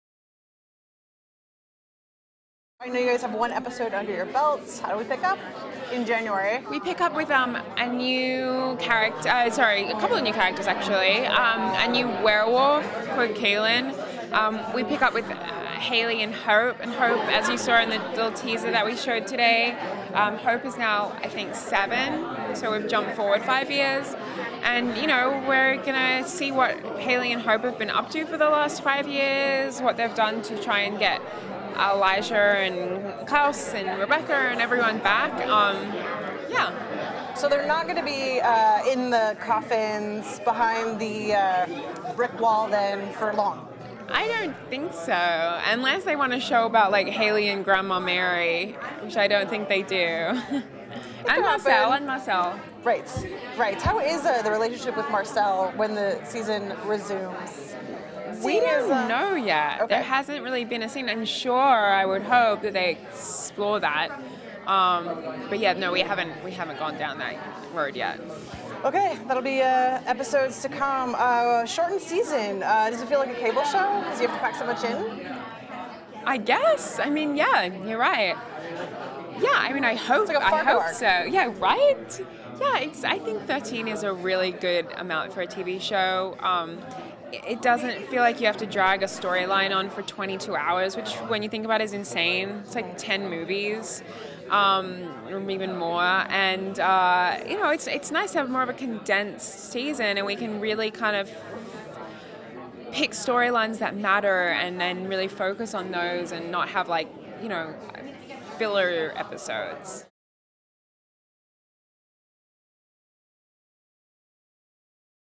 'The Originals': Phoebe Tonkin Talks Time Jump, New Werewolf & More | Access Hollywood
Phoebe Tonkin, who plays Hayley on The CW's "The Originals," tells Access Hollywood about her show's five-year time jump when it returns in 2017.